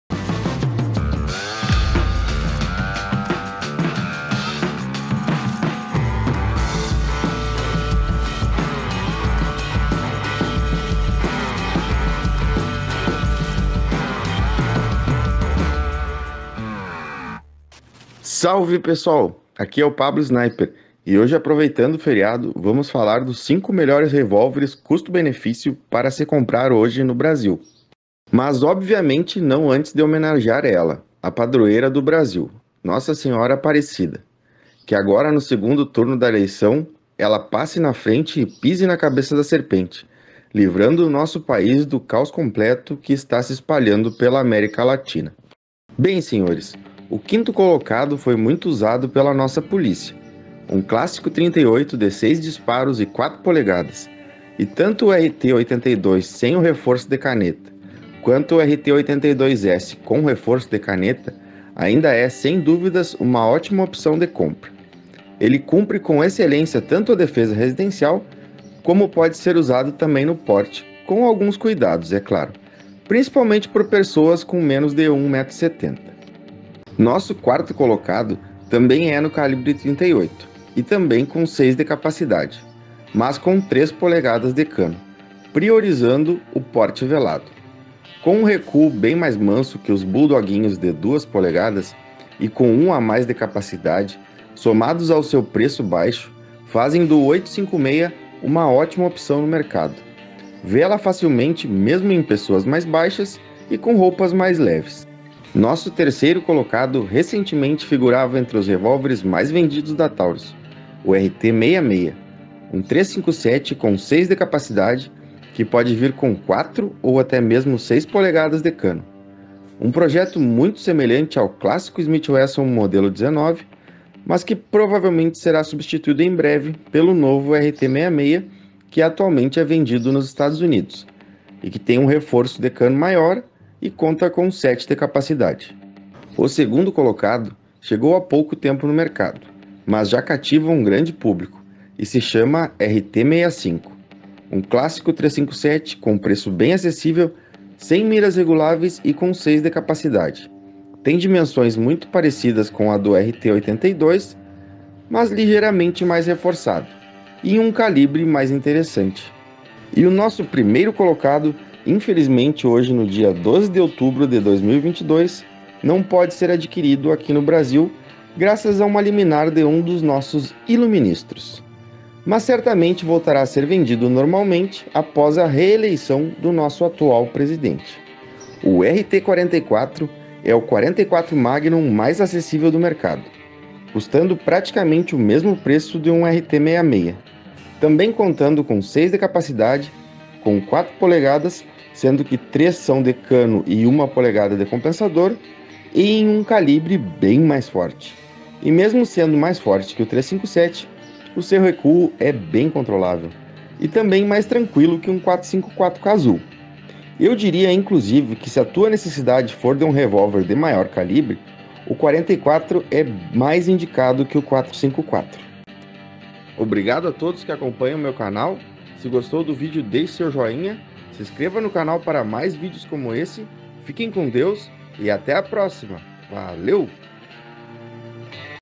Top 5 Revolver Taurus custo benefício 2022
Pra quem é revolveiro raiz, não poderá perder esse vídeo!